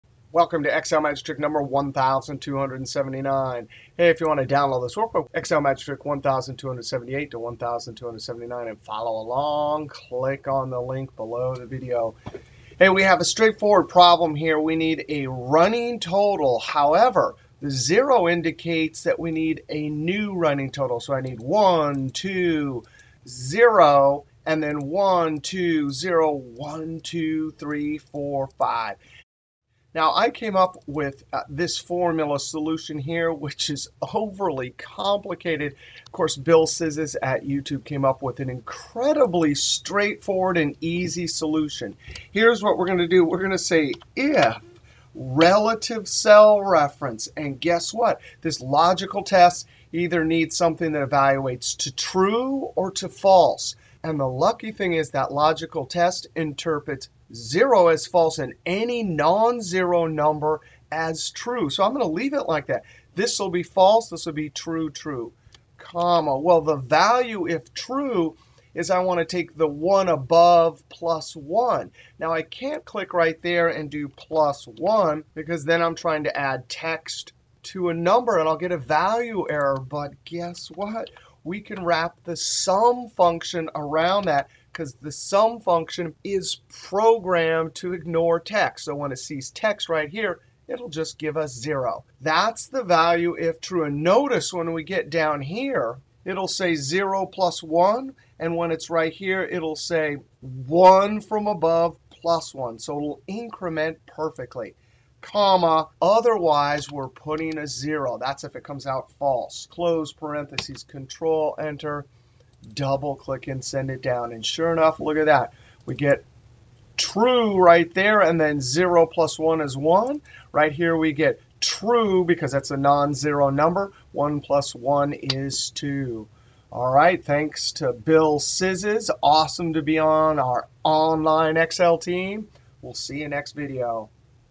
Excel Magic Trick 1279: New Running Count Formula After Each Zero Value (Cumulative Count)
Learn how to create a New Running Count (Cumulative Count) Formula After Each Zero Value using the IF and SUM functions. Trick from Bill Szysz at YouTube. See that the SUM Function ignores Text Values.